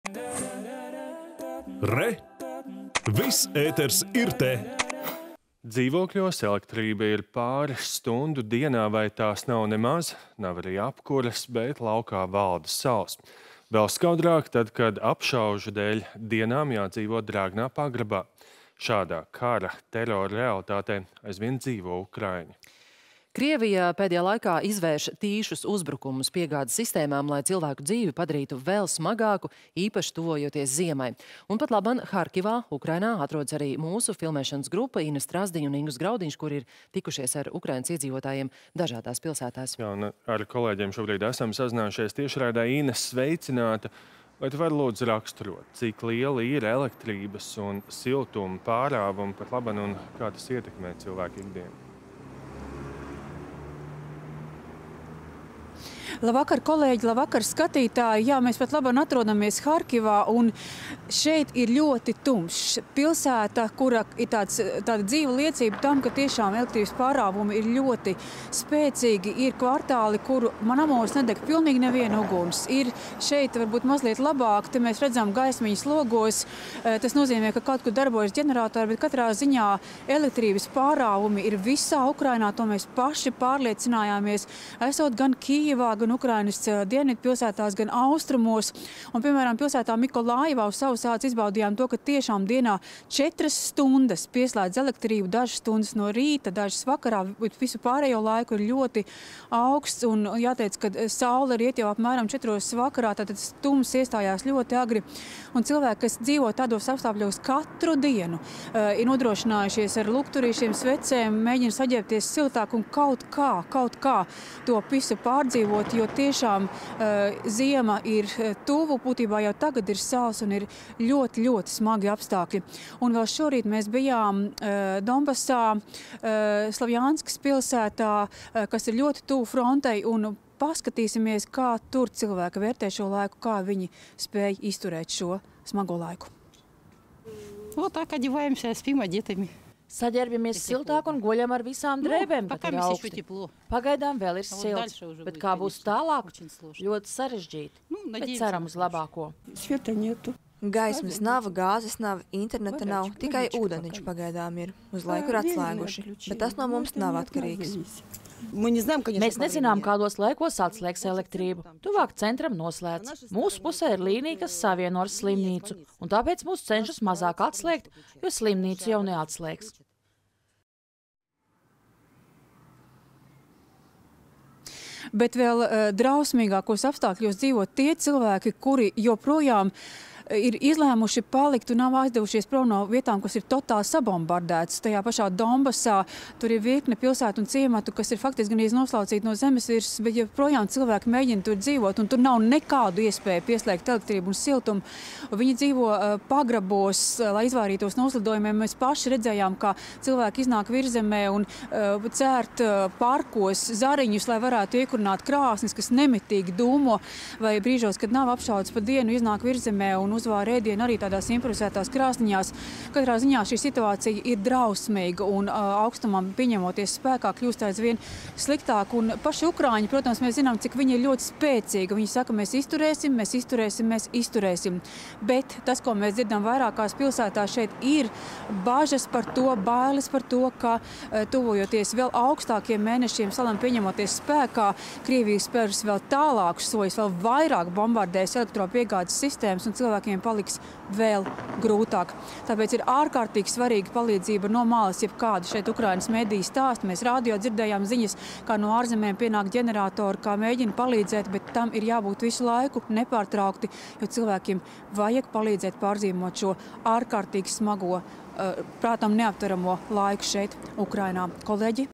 Ukrainā iztur karu bez siltuma un elektrības
Dzīvokļos elektrība ir pāris stundu dienā vai tās nav nemaz, nav arī apkures, bet laukā valda sals. Vēl skaudrāk tad, kad apšaužu dēļ dienām jādzīvo drēgnā pagrabā. Šādā kara terora realitātē dzīvo ukraiņi. Krievija pēdējā laikā izvērš tīšus uzbrukumus piegādes sistēmām, lai cilvēku dzīvi padarītu vēl smagāku, īpaši tuvojoties ziemai. Un patlaban Harkivā, Ukrainā, atrodas arī mūsu filmēšanas grupa – Ina Strazdiņa un Ingus Graudiņš, kuri ir tikušies ar Ukrainas iedzīvotājiem dažādās pilsētās.